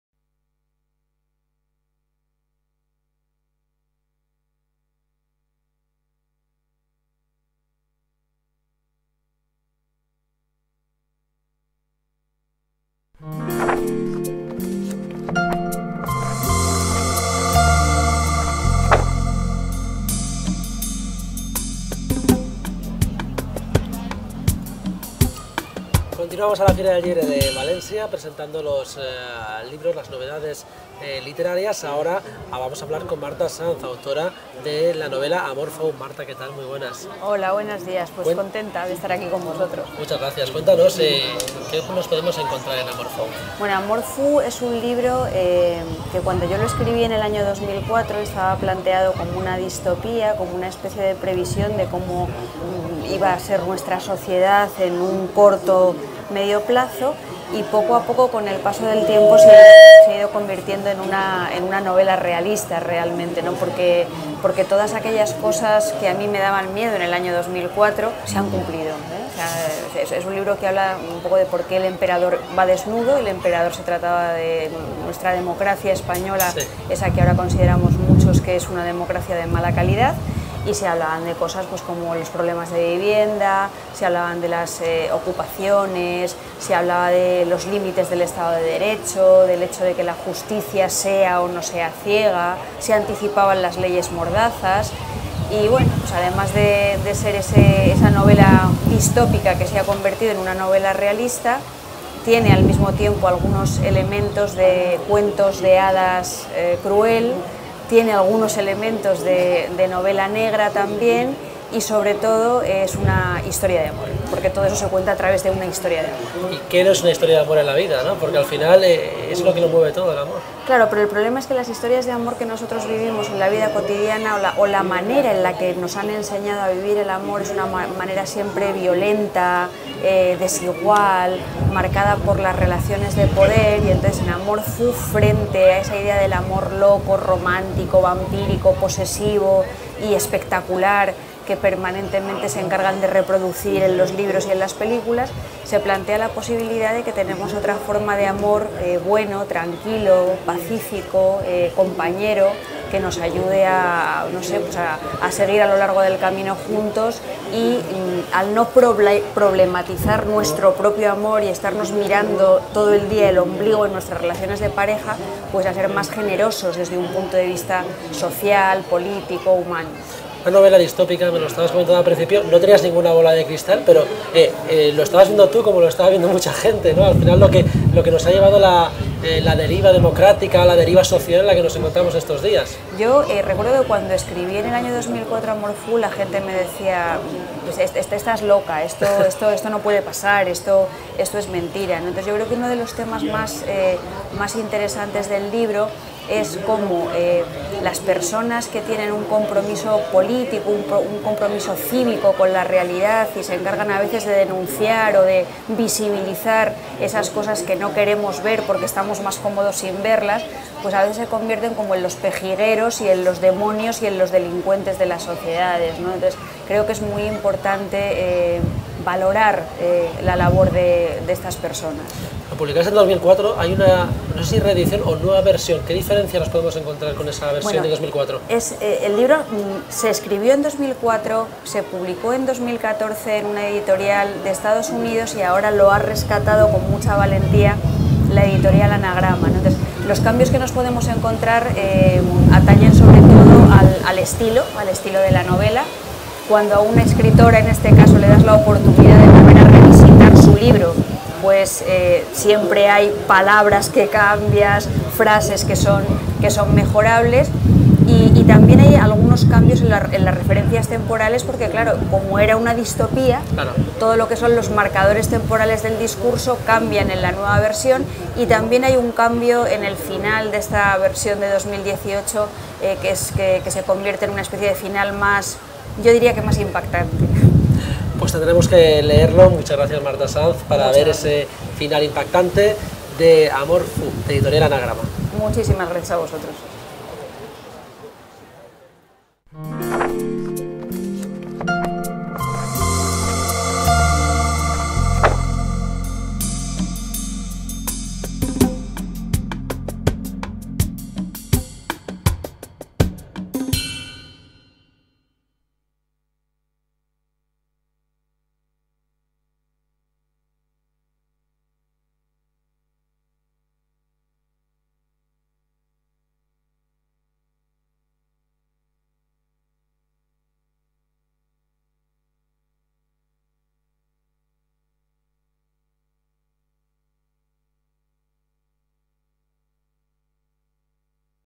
Fira del Llibre 2018 - MARTA SANZ "Amor fou"
Este es el libro por el que Marta Sanz estuvo a punto de dejar de escribir. Una novela prácticamente inédita, una novela sin lectores, tal vez porque habla del gusano que corroe el corazón de la manzana en un mundo perfecto. En 2004, año en que fue escrita, Amor fou apuntaba hacia lo mucho que nos cuesta decir que el emperador va desnudo; lo hace también hoy, que la presentamos en versión corregida y actualizada. Casi todas las profecías de esta novela se han ido cumpliendo: aporofobia, gentrificación, banderas nacionales que ondean en el centro de las plazas, un patriotismo perturbado, el residuo franquista que oxida la convivencia, la brutalidad que se ejerce desde el poder, la okupación, los límites de la democracia y del Estado de derecho en el neoliberalismo, la justicia sin venda en los ojos, la manipulación pública a la que se someten ciertas vidas íntimas... La ponzoña es la metáfora que nutre una escritura de profundidad espeleológica.

Amor fou plantea preguntas en torno a nuestra educación sentimental y política. El amor empasta las voces, y la literatura se aparta de la suavidad deslizante de la seducción, para subrayar su violencia. La mirada del Marqués de Sade más educativo envenena las manzanas y el alimento de Los emperadores. Porque posiblemente Amor fou es un cuento de hadas salvaje, de esos que se censuran para no escandalizar a los niños, ni a los adultos que preferirían permanecer en una infancia eterna.

En esta historia triangular Raymond, desde su observatorio, vigila la felicidad conyugal de Adrián y Lala, su antigua novia, y no puede soportarla. Decide intervenir en ella con su mirada evocadora y su presencia disfrazada. Raymond lleva una barba postiza. Pero hasta las pequeñas maldades pueden tener horrendos efectos secundarios. El peligro se hace más intenso cuando Elisa y su hija Esther, cebada como esos niños canibalizados por la bruja, interfieren en la historia y traen con ellas los incendios, los anónimos, el abuso, las cicatrices y una ridícula caja de bombones envenenados.

«El amor como posibilidad llena de trampas, el amor como dolor, como enfermedad y locura. Puede que sea una de las novelas más dolorosas de Marta Sanz, y eso es mucho dolor, aunque sea por nuestro bien: Sanz nunca nos causa daño en vano, las heridas que deja son una forma de lucidez. Sirven» (Isaac Rosa).